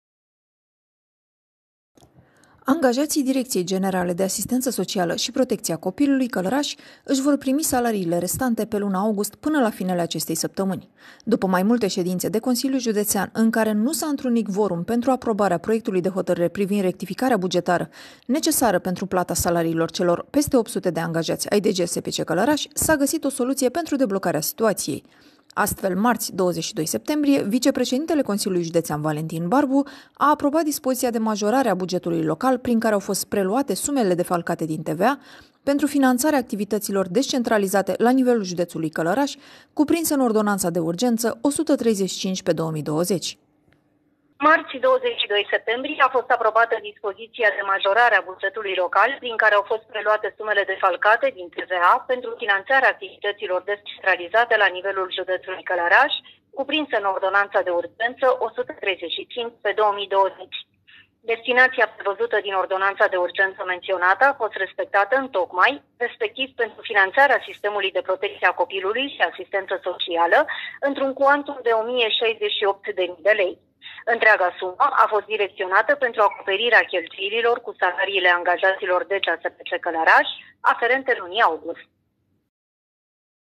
Angajații DGASPC își vor primi salariile
Angajații Direcției Generale de Asistență Socială și Protecția Copilului (DGASPC) Călărași își vor primi salariile restante pe luna august până la finele acestei săptămâni.